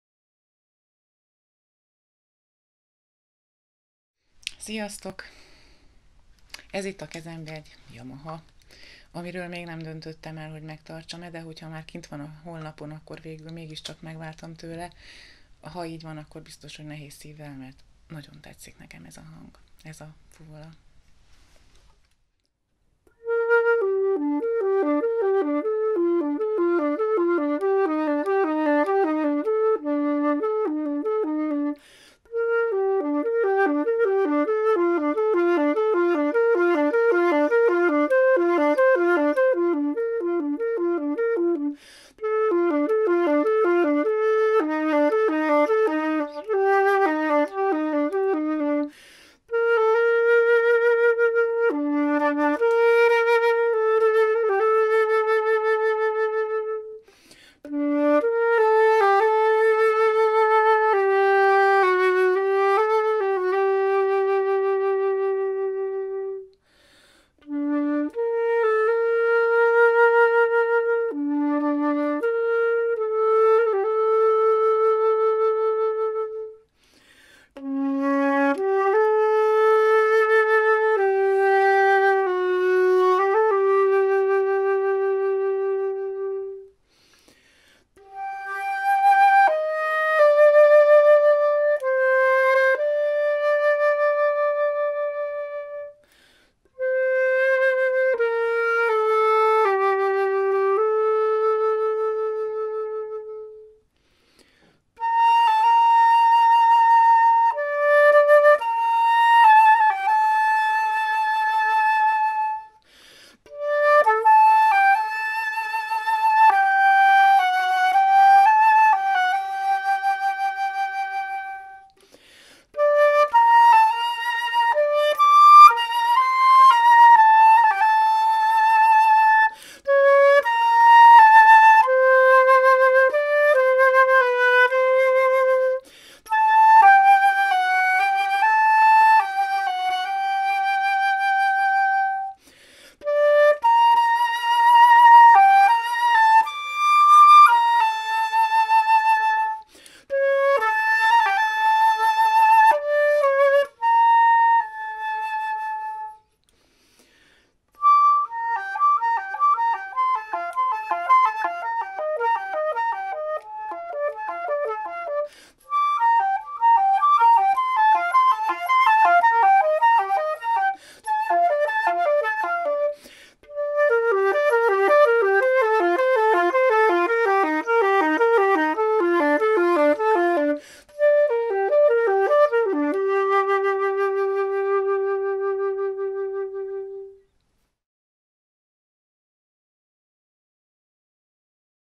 A hasznaltfuvola.hu bemutatja: no.116 Yamaha 211 SII
Gyönyörű hangú fuvola, szeretettel ajánlom figyelmetekbe